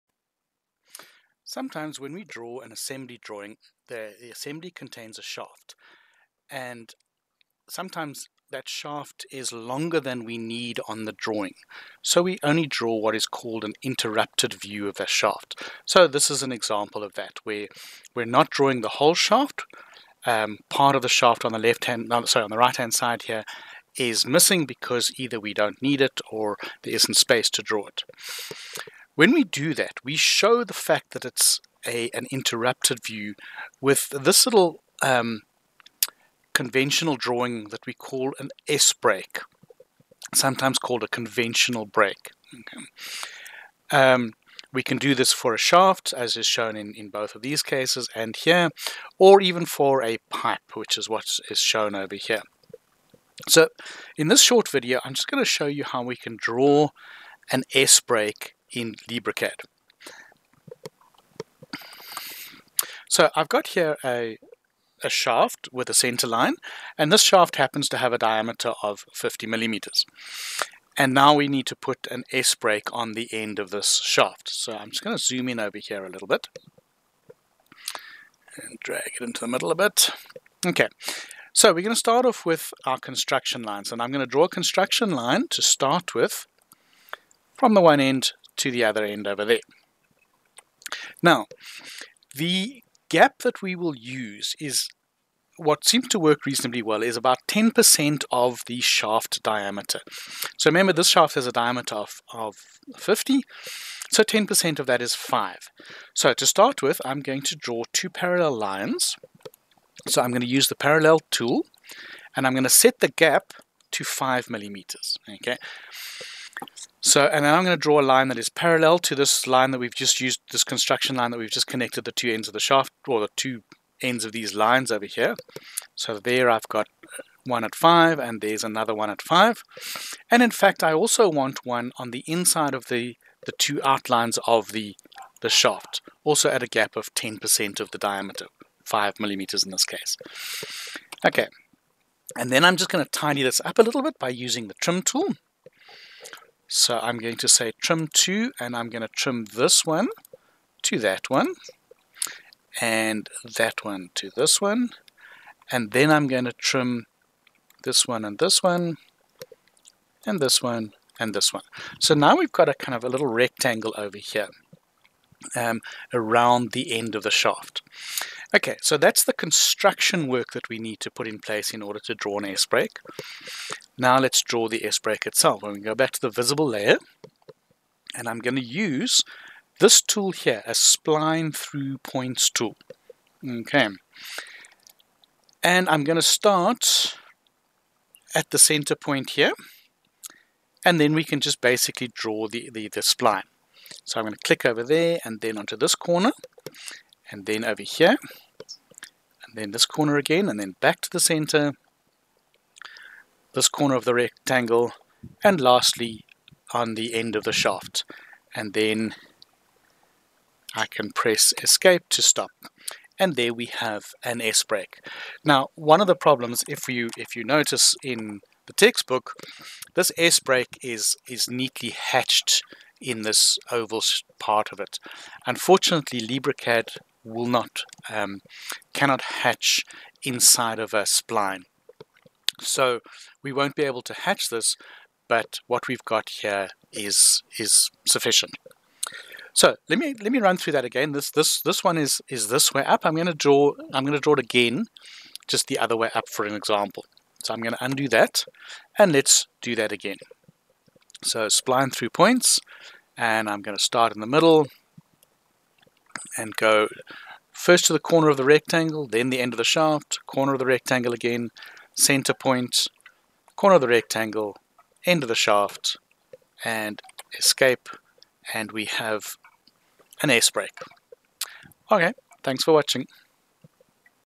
Drawing an s-break
This video demonstrates how to draw an s-break, also called a conventional break, in LibreCAD.